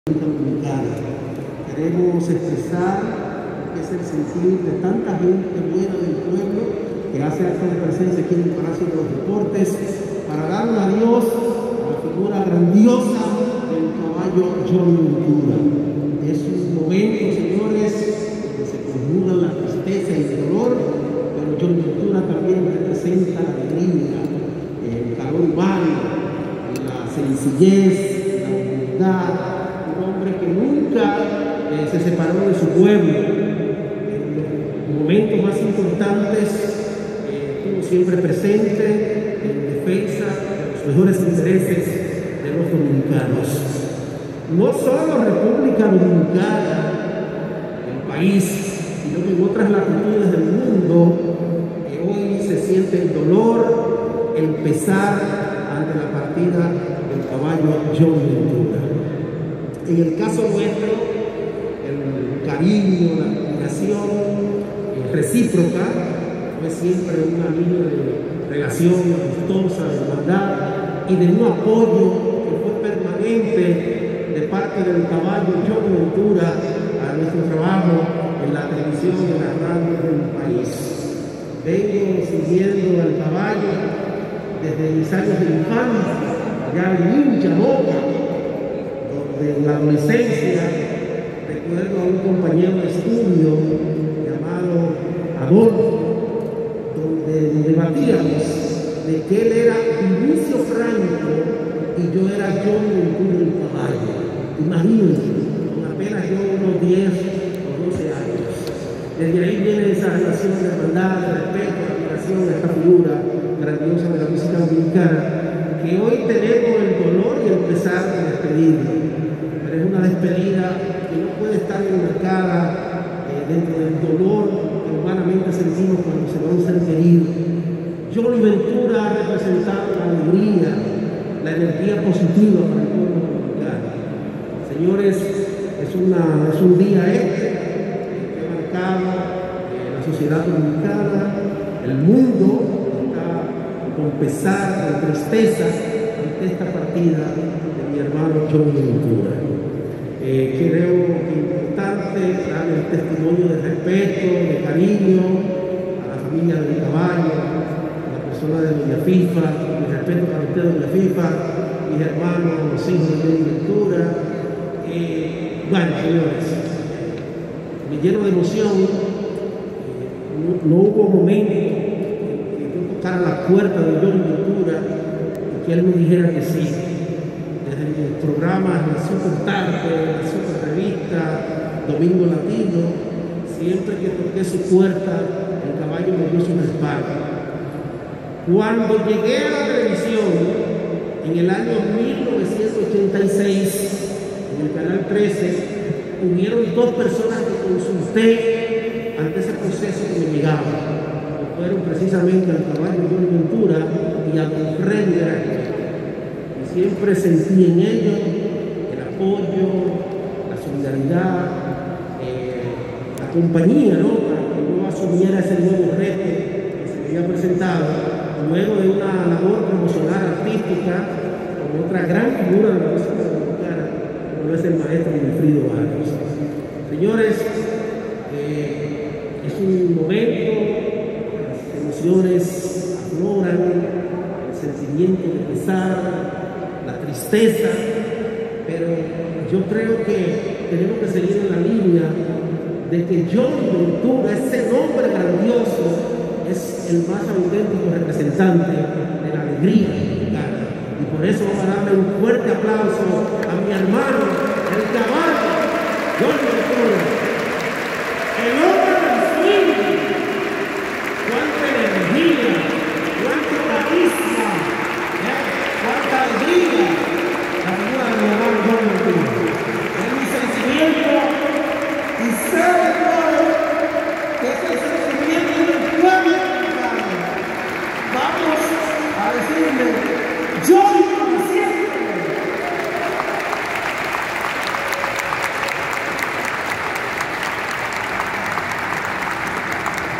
Despedida a Johnny Ventura en el Palacio de los Deportes
El comunicador Domingo Bautista mientras da pésame a la familia Ventura, en el acto de despedida de El Caballo Mayor, el merenguero  Johnny Ventura.